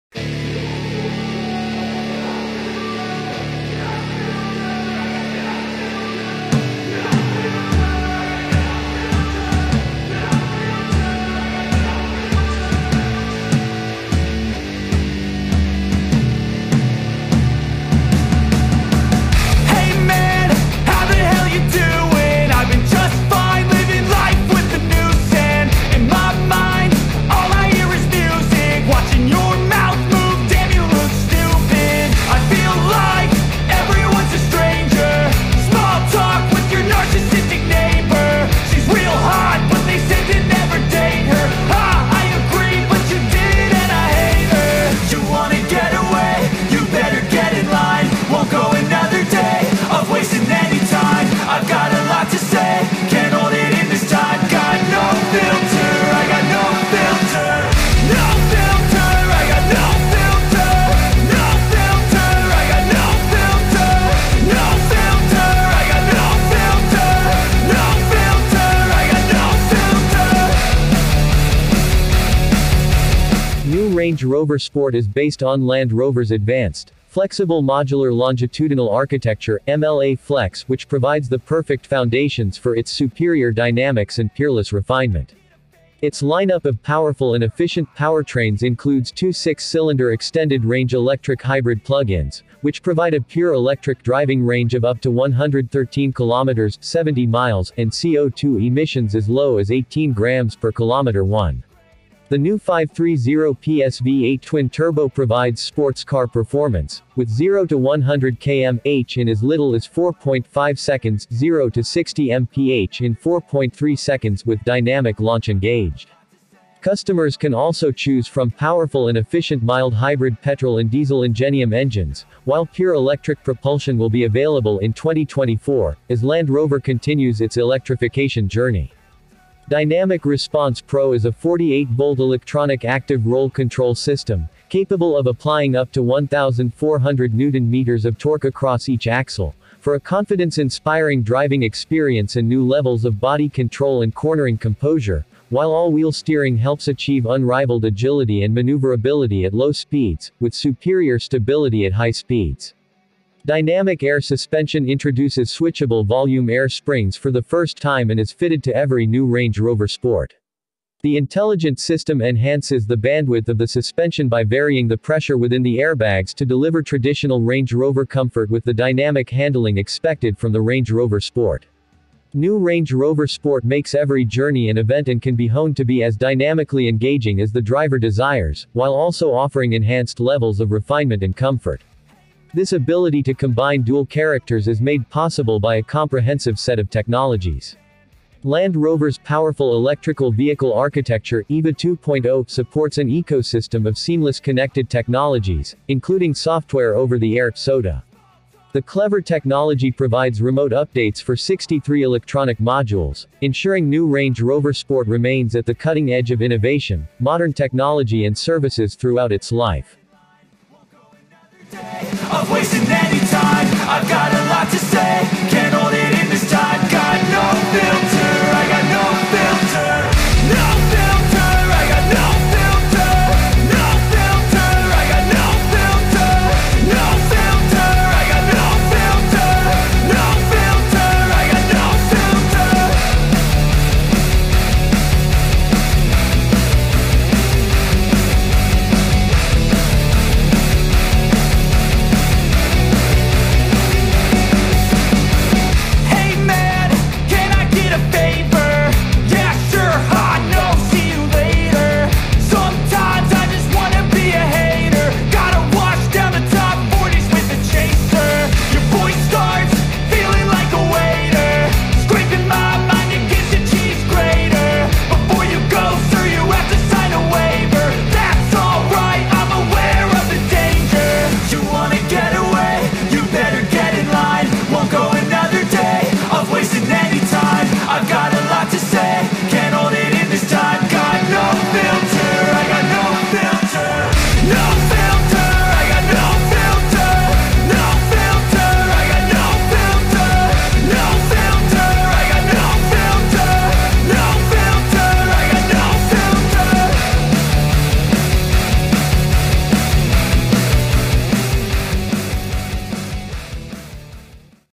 New Range Rover Sport - Powerful and Efficient Powertrains Electric Vehicle
New Range Rover Sport is based on Land Rover’s advanced, flexible Modular Longitudinal Architecture (MLA-Flex), which provides the perfect foundations for its superior dynamics and peerless refinement. Its line-up of powerful and efficient powertrains includes two six-cylinder extended range Electric Hybrid plug-ins, which provide a pure-electric driving range of up to 113km (70 miles) and CO2 emissions as low as 18g/km1.

The new 530PS V8 Twin Turbo provides sports car performance, with 0-100km/h in as little as 4.5 seconds (0-60mph in 4.3 seconds) with Dynamic Launch engaged. Customers can also choose from powerful and efficient mild hybrid petrol and diesel Ingenium engines, while pure-electric propulsion will be available in 2024, as Land Rover continues its electrification journey.